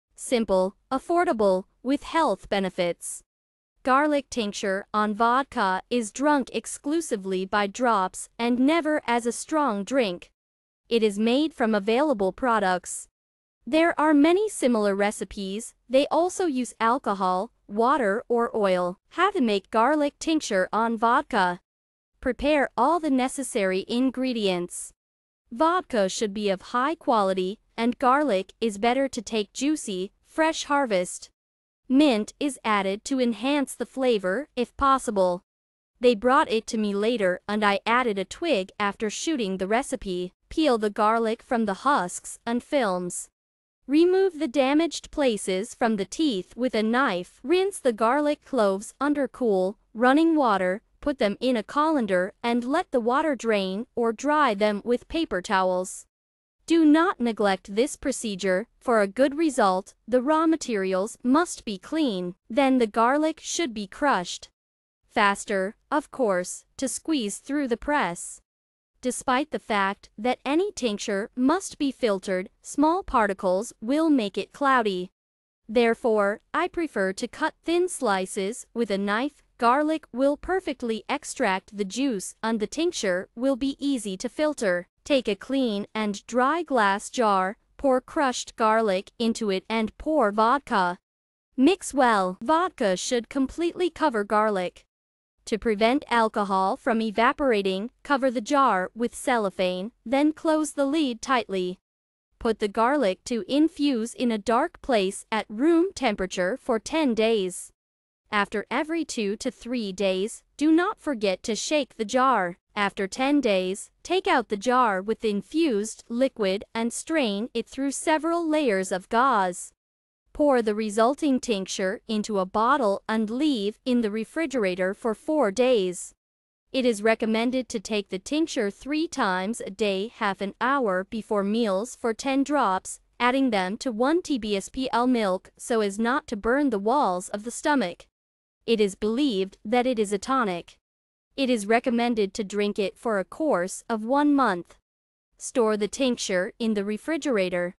Tincture of garlic on vodka
Simple, affordable, with health benefits! Garlic tincture on vodka is drunk exclusively by drops and never as a strong drink. It is made from available products. There are many similar recipes, they also use alcohol, water or oil.

Ingredients:
Garlic - 200 g, Vodka - 300 ml,
Mint - 10 pcs., Garlic - 200 gr,
Vodka - 300 ml, Mint - 10 pcs.,